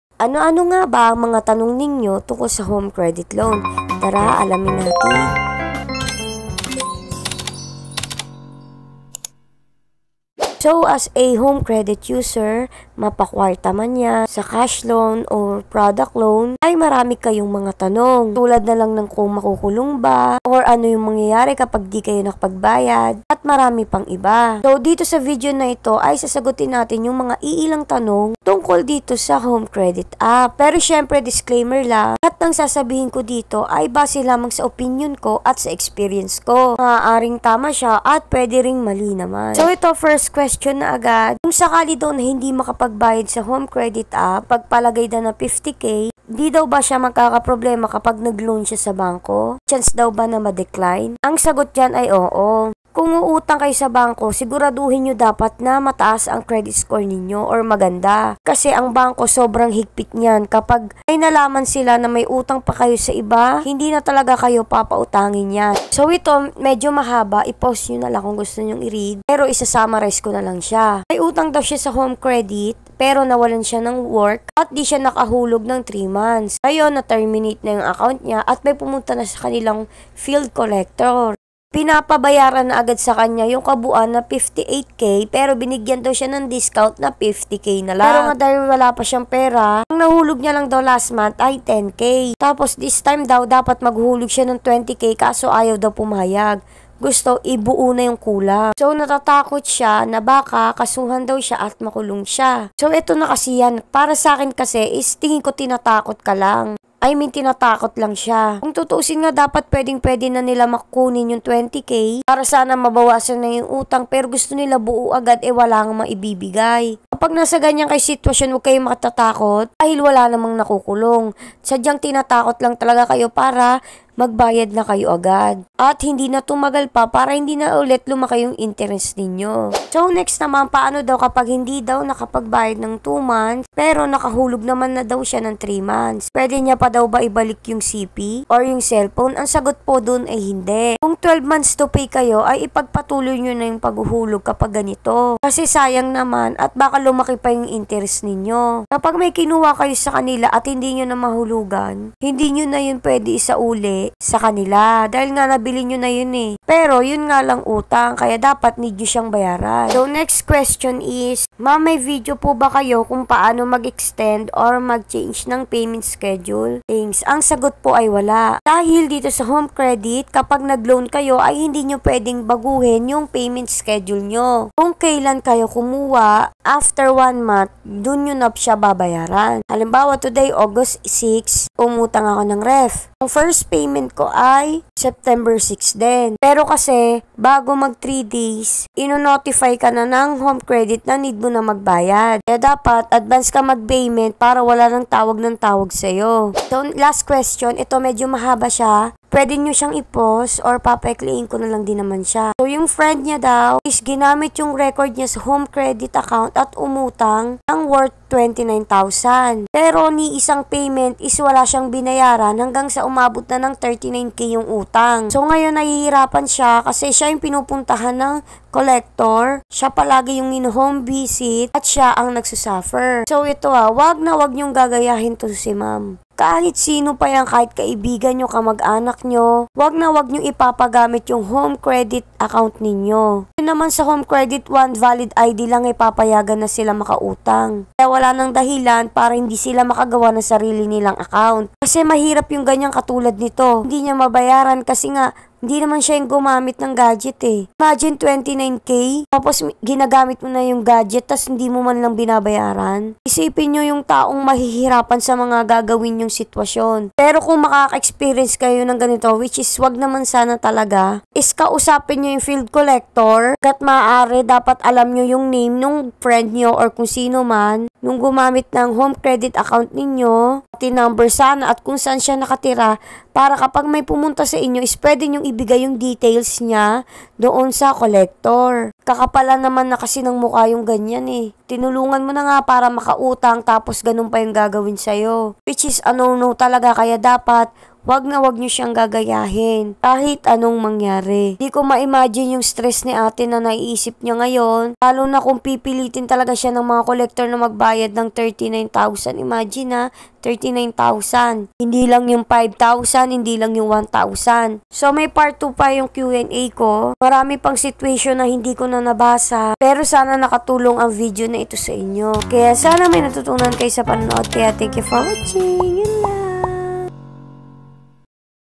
MAKUKULONG BA KAPAG DI NAGBAYAD SA HOME CREDIT? Q AND A ABOUT HOME CREDIT LOAN
Tanong mo, Sagot ko!